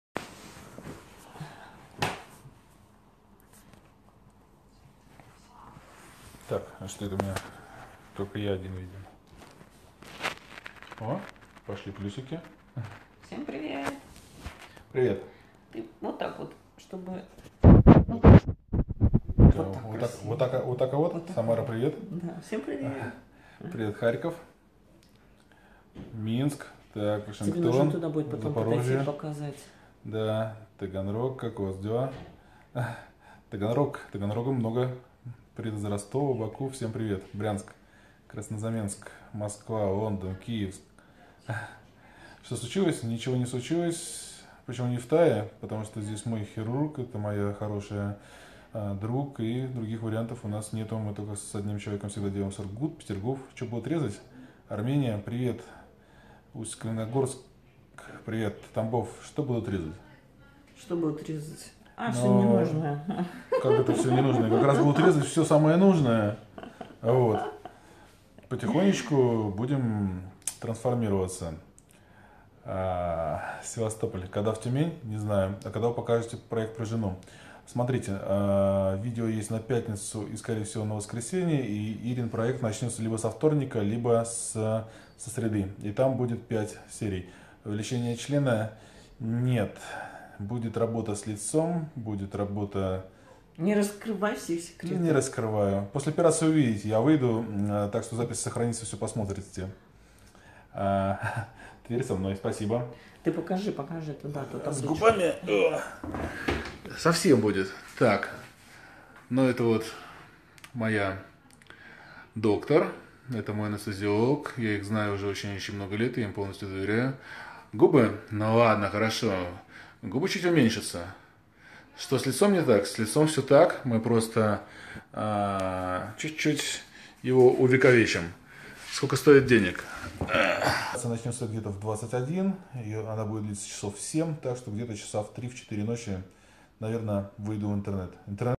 Саша Шпак попал в больницу
Александр Шпак снова решил себя изменить и это точно не в последний раз